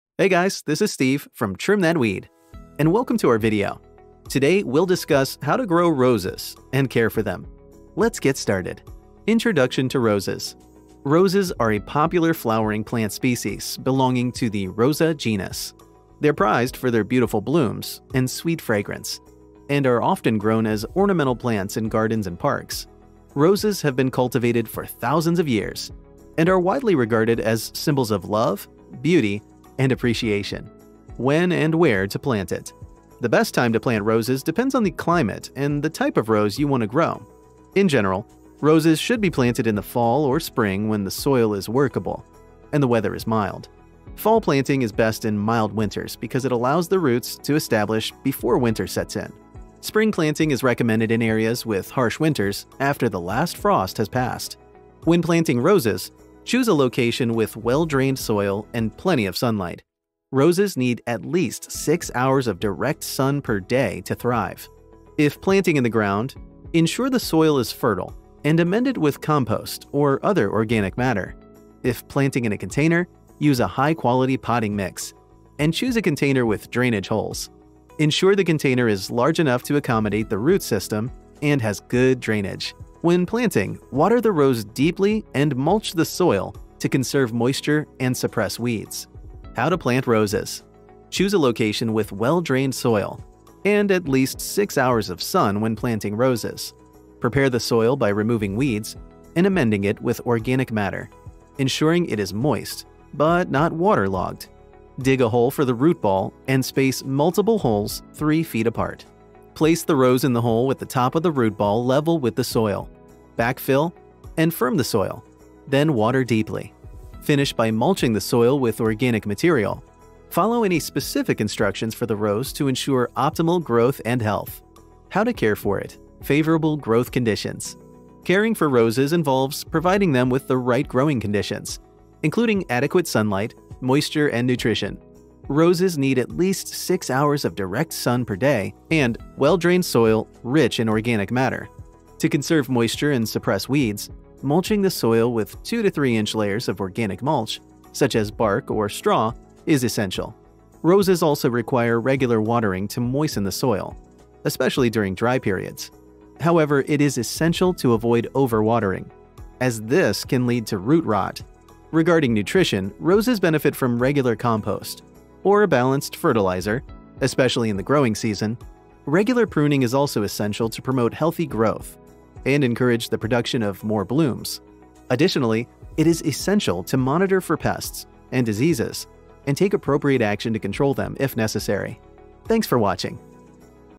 Rose – How to grow and care for it
Rose – How to grow and care for it. Welcome to our video. Today, we will discuss how to grow Roses and care for them.

Introduction to Rose
Roses are a popular flowering plant species belonging to the Rosa genus. They are prized for their beautiful blooms and sweet fragrance and are often grown as ornamental plants in gardens and parks. Roses have been cultivated for thousands of years and are widely regarded as symbols of love, beauty, and appreciation.

How to plant Roses
Choose a location with well-drained soil and at least 6 hours of sun when planting roses. Prepare the soil by removing weeds and amending it with organic matter, ensuring it is moist but not waterlogged. Dig a hole for the root ball and space multiple holes 3 feet apart.
Place the rose in the hole with the top of the root ball level with the soil, backfill, and firm the soil, then water deeply. Finish by mulching the soil with organic material. Follow any specific instructions for the rose to ensure optimal growth and health.